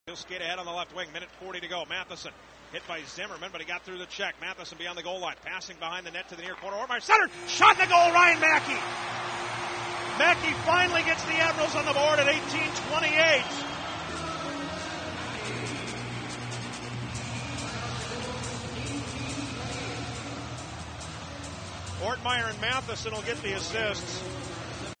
Ryan Maki scores the Admirals lone goal of the night
Ryan Maki of the Milwaukee Admirals scores from close range on 1/23/09 against the San Antonio Rampage.